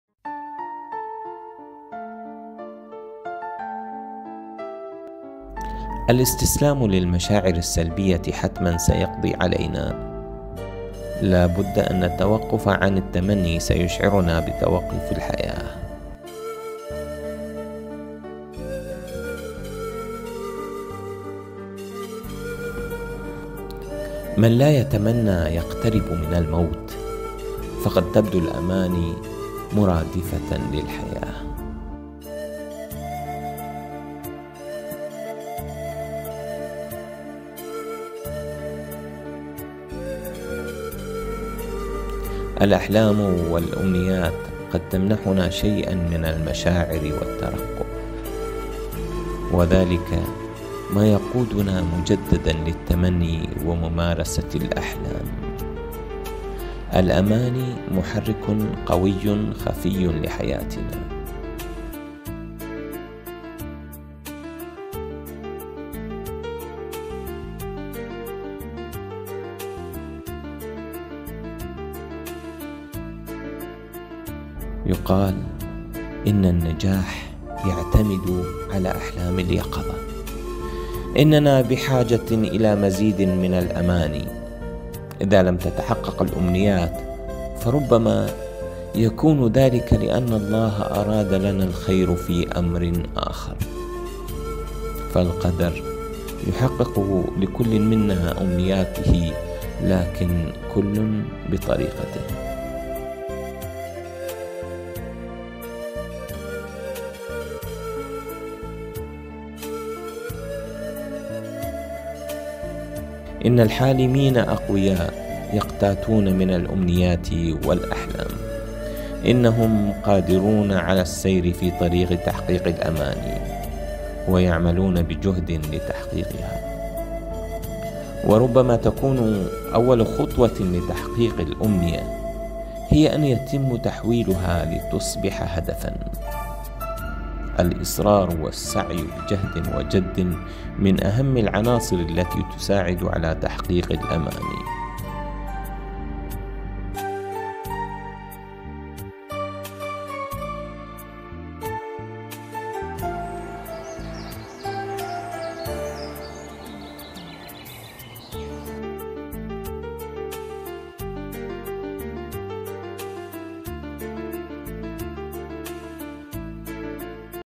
تحقيق الاماني.. سعيد البادي
تحقيق الأماني.. سعيد البادي
الاستسلام للمشاعر السلبية حتماً سيقضي علينا، لا بد أن التوقف عن التمني سيُشعرنا بتوقف الحياة، من لا يتمنى يقترب من الموت، فقد تبدو الأماني مرادفة للحياة، الأحلام والأمنيات قد تمنحنا شيئاً من المشاعر والترقب، وذلك ما يقودنا مجدداً للتمني وممارسة الأحلام، الأماني محرك خفي لحياتنا، يقال إن النجاح يعتمد على أحلام اليقظة، إننا بحاجة إلى مزيد من الأماني، إذا لم تتحقق الأمنيات فربما يكون ذلك لأن الله أراد لنا الخير في أمر آخر، فالقدر يحقق لكل منا أمنياته لكن كل بطريقته.
إن الحالمين أقوياء، يقتاتون من الأمنيات والأحلام، إنهم قادرون على السير في طريق تحقيق الأماني، ويعملون بجهد لتحقيقها، وربما تكون أول خطوة لتحقيق الأمنية هي أن يتم تحويلها لتصبح هدفاً، الإصرار والسعي بجهد وجد من أهم العناصر التي تساعد على تحقيق الأماني.